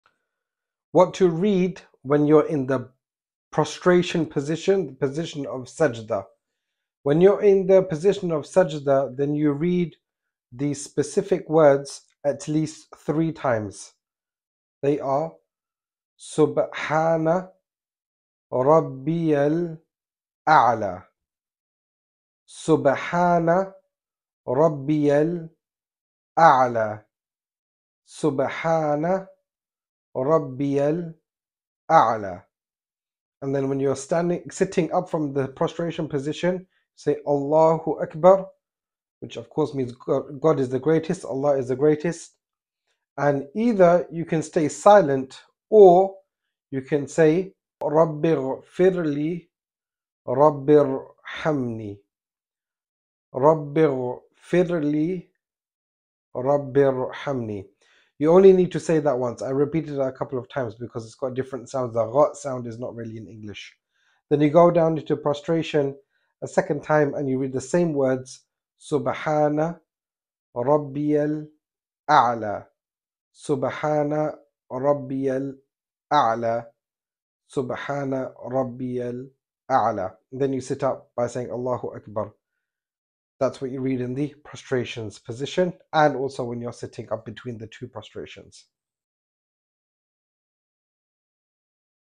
What to Read in Sajdah (Prostration Position) in Salah Prayer
In this video I explain what to read in Salah Sajdah position.